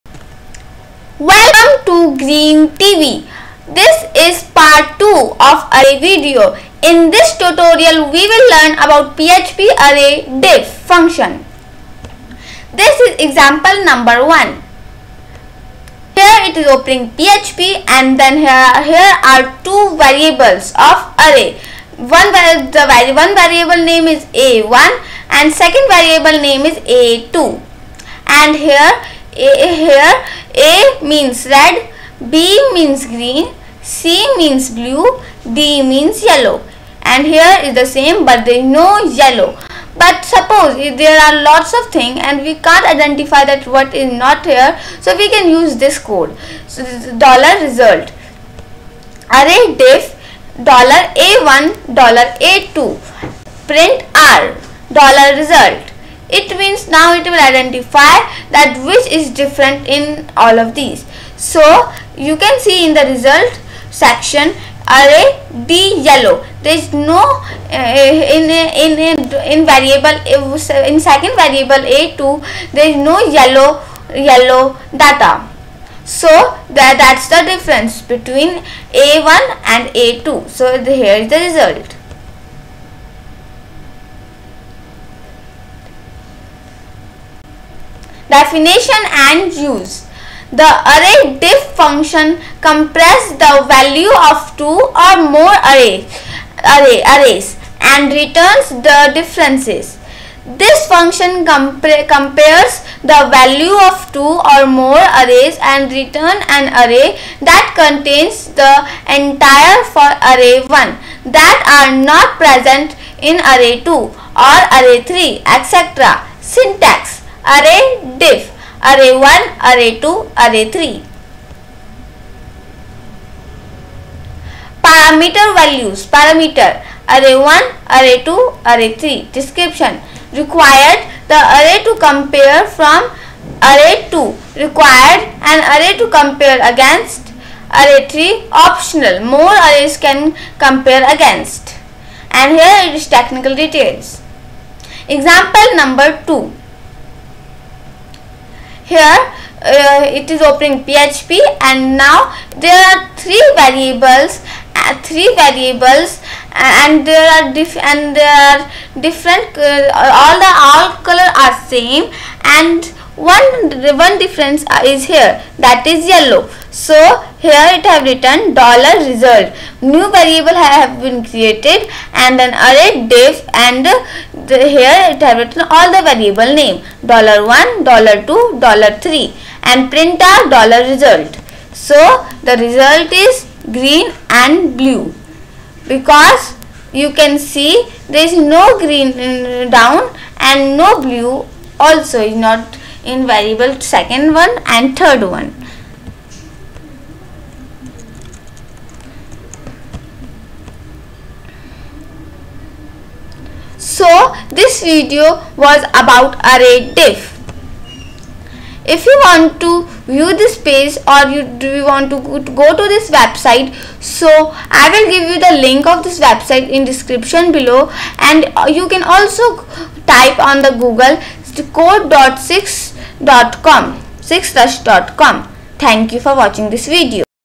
Array_Diff in PHP in english Part 2
PHP array_diff() Function

Definition and Usage
The array_diff() function compares the values of two (or more) arrays, and returns the differences.

This function compares the values of two (or more) arrays, and return an array that contains the entries from array1 that are not present in array2 or array3, etc.